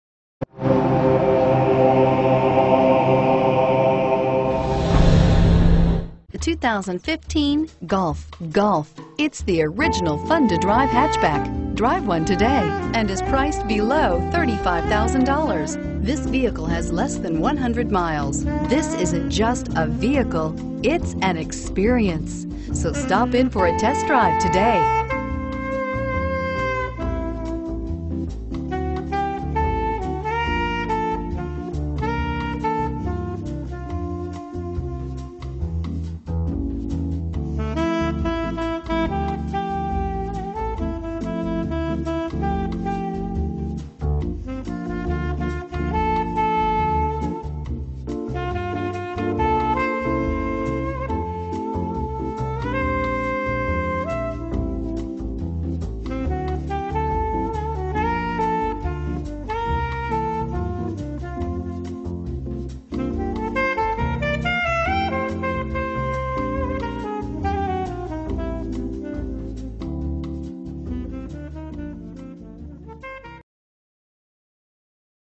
2015 Volkswagen Golf SportWagen Valencia CA 2159410
2015 Volkswagen Golf SportWagen TDI SEL

Parkway Motorcars
24075 Magic Mountain Parkway

Don't miss this 2015 Volkswagen Golf SportWagen. It's equipped with Automatic transmission and features a 2B2B exterior. With 50 miles, You'll want to take this  home. Make a great choice today.